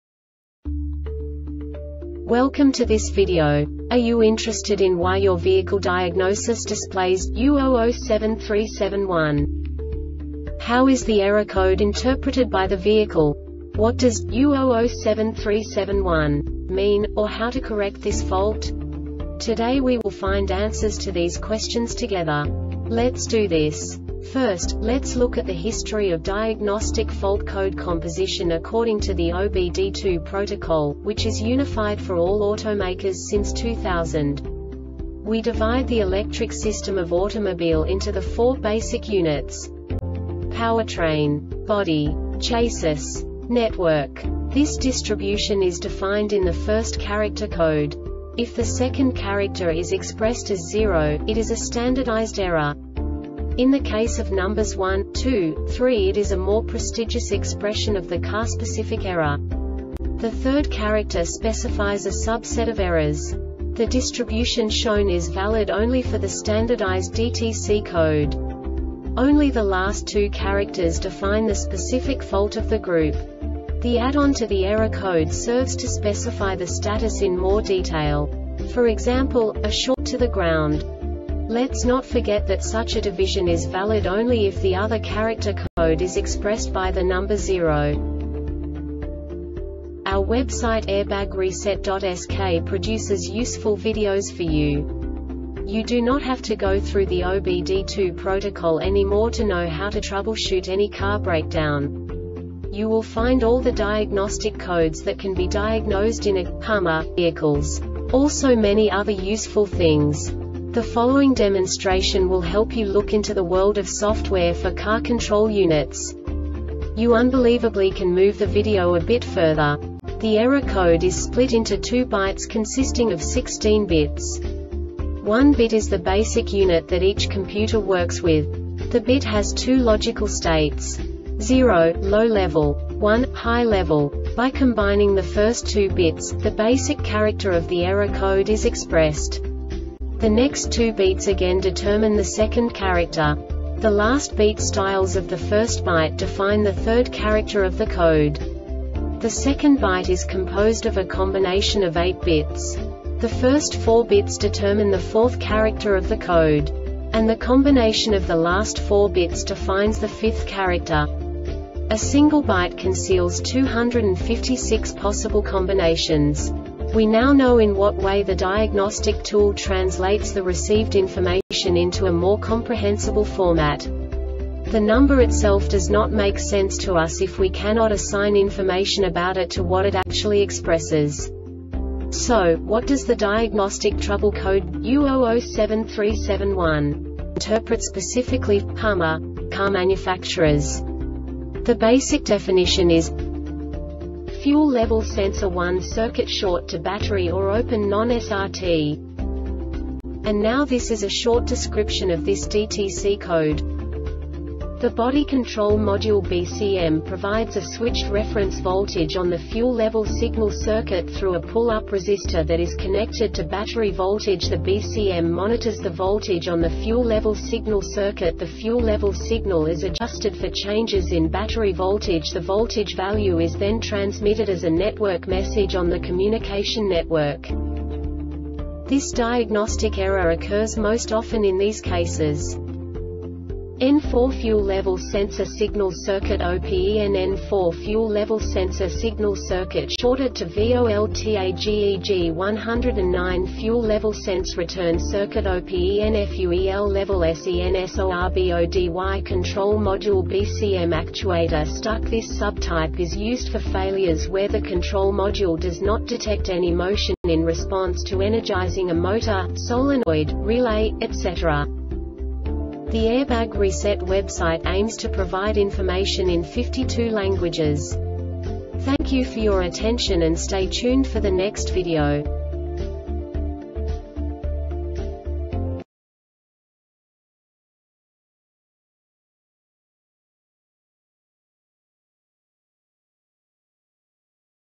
DTC Hummer U0073-71 Short Explanation
Contents:
0:21 Basic DTC analysis according to OBD2 protocol standard.
1:48 Insight into programming
2:58 A diagnostically understandable description of the Diagnostic Trouble Code
3:05 Basic error definition

Make:
HUMMER
Code:
U0073 71
Definition:
Fuel Level Sensor 1-Circuit Short To Battery Or Open-Non SRT
Description:
The Body Control Module (BCM) provides a switched reference voltage on the fuel level signal circuit through a pull-up resistor that is connected to battery voltage. The BCM monitors the voltage on the fuel level signal circuit. The fuel level signal is adjusted for changes in battery voltage. The voltage value is then transmitted as a network message on the communication network.
Cause:
(N4) FUEL LEVEL SENSOR SIGNAL CIRCUIT OPEN(N4) FUEL LEVEL SENSOR SIGNAL CIRCUIT SHORTED TO VOLTAGE(G109) FUEL LEVEL SENSE RETURN CIRCUIT OPENFUEL LEVEL SENSORBODY CONTROL MODULE (BCM)
Failure Type:
Actuator Stuck